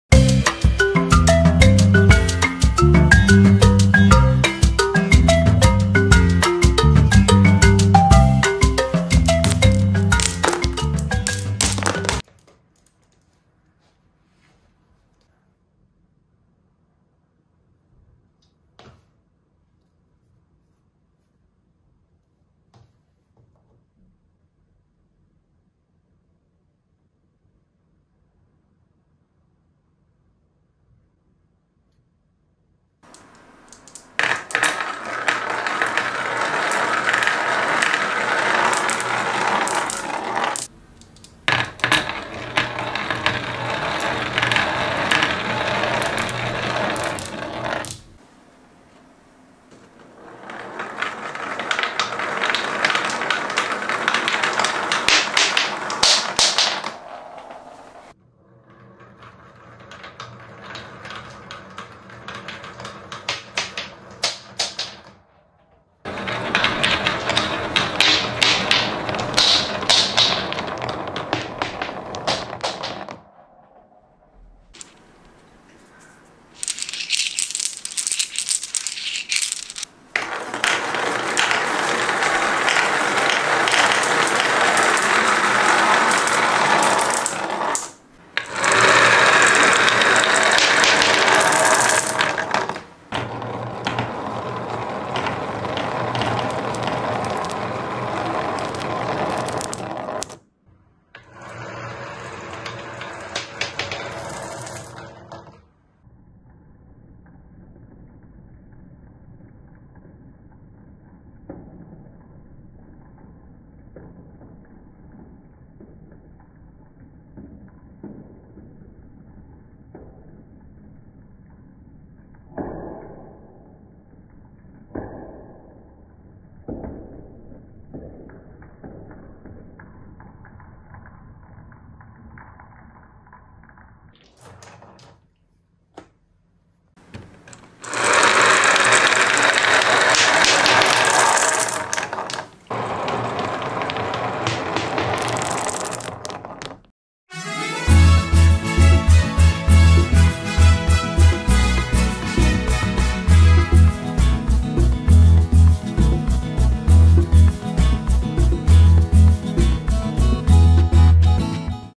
Marble Run Race ASMR - Sand & Satisfying Marble Run Volcano
Relaxing sounds of the marbles rolling over sand and wood tracks. ASMR satisfying videos with sand, wooden, water and home made marble run races. We hope you enjoy this video.